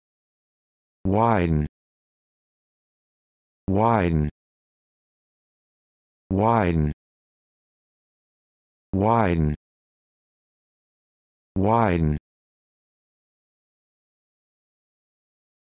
How to Pronounce Widen
This video shows you how to pronounce Widen